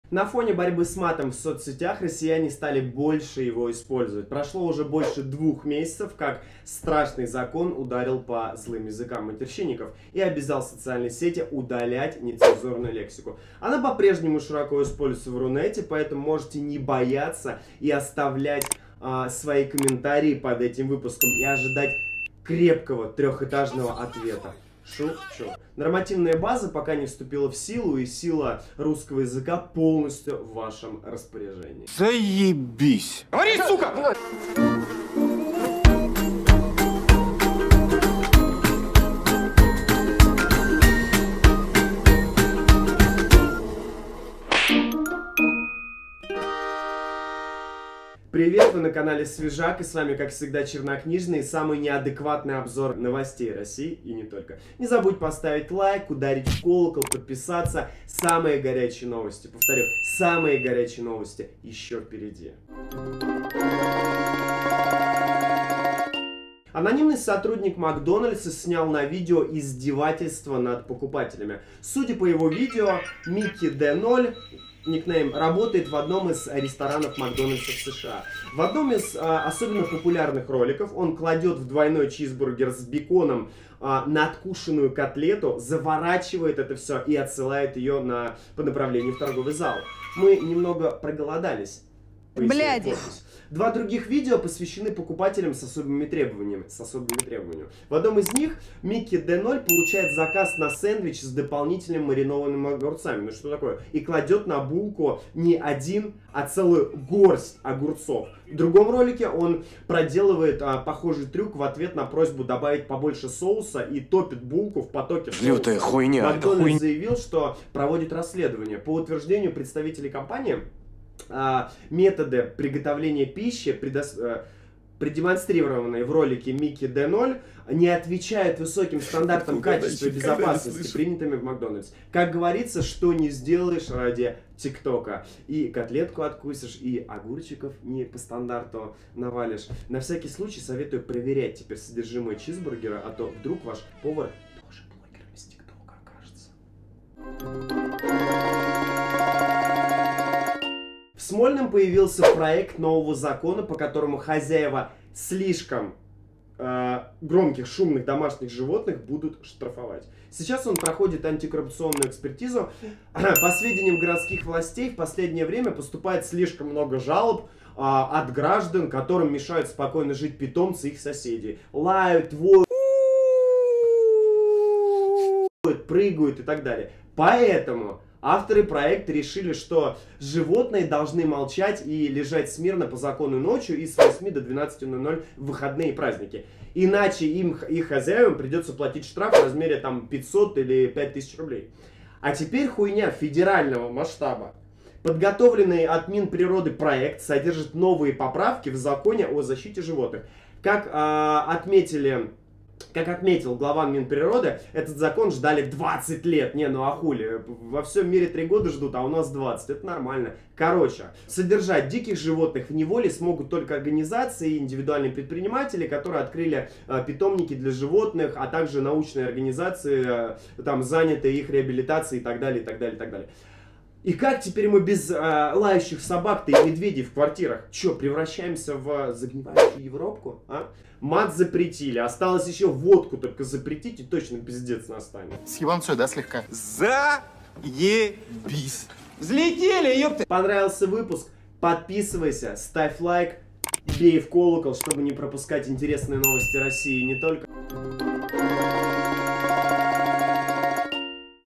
Запрет на мат не работает / Собакам запрещают лаять / Тикток в Макдоналдс / Чернокнижный
Запрет на мат не работает. Собакам запрещают лаять. Тикток в Макдоналдс. Чернокнижный обзор интересных новостей России и не только!

Тайм-коды:

00:00 Подписывайся на СВЕЖАК

00:15 - Мат в социальных сетях
01:08 - Издевательство над покупателем
02:30 - Собакам запретили лаять
04:02 - Подписывайся на СВЕЖАК